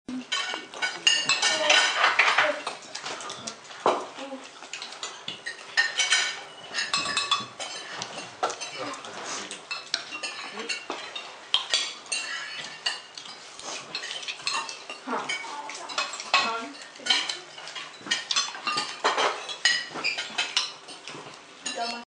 저녁식사 dinner
언제나 밍밍한 서양음식을 먹는 아이들은 한국의 칼칼한 맛이 나는 음식을 그리워한다. 그래서 갈비찜을 하면서 일반적인 간장맛으로 간을 한 갈비찜이 아니라 매칼한 태양초 고추기름으로 맛을 낸 갈비찜 육개장을 해줬다.